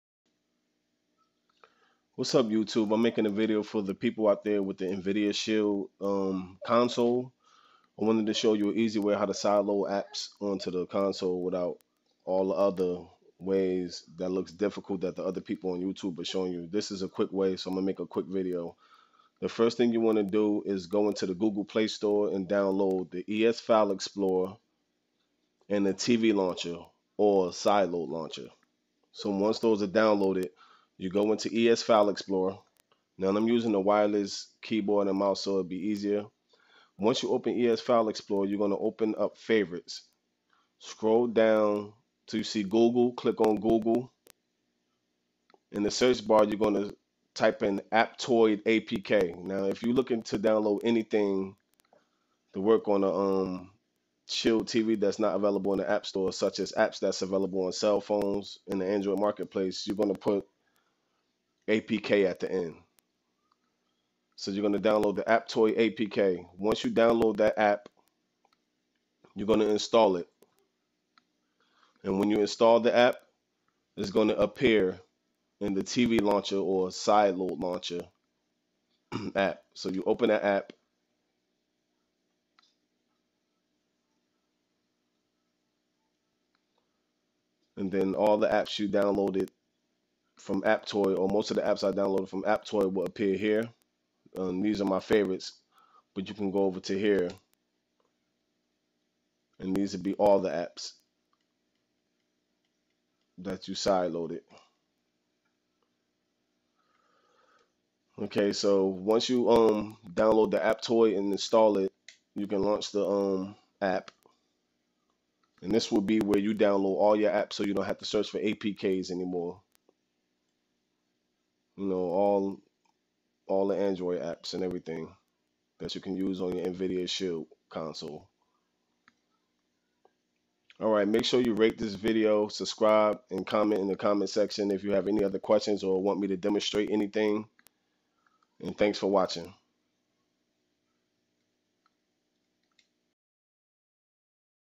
The fastest & easiest way to sideload apps to the Nvidia Shield console
A quick video on how to side load apps to the Nvidia Shield